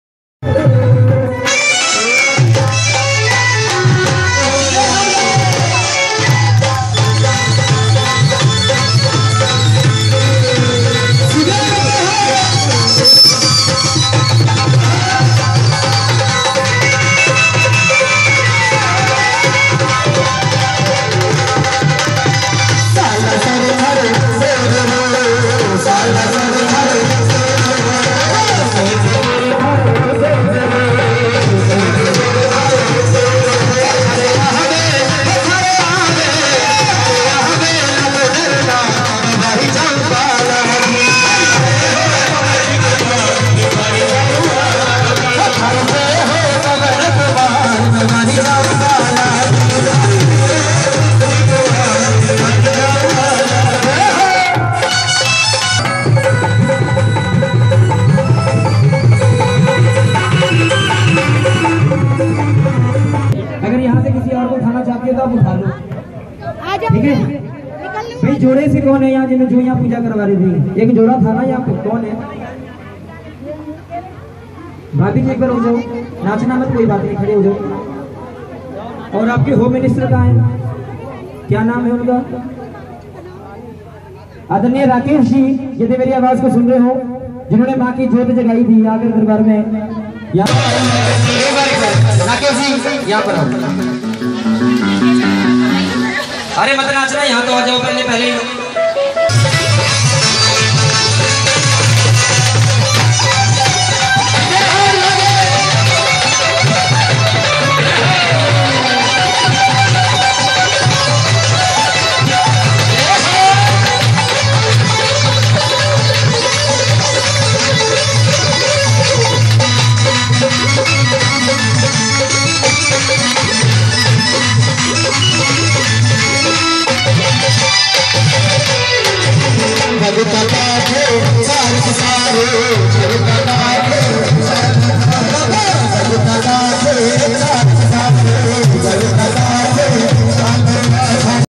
Jagaran At Arjun Park, Nagli
Dance by Acharya Nitesh Sundriyal Ji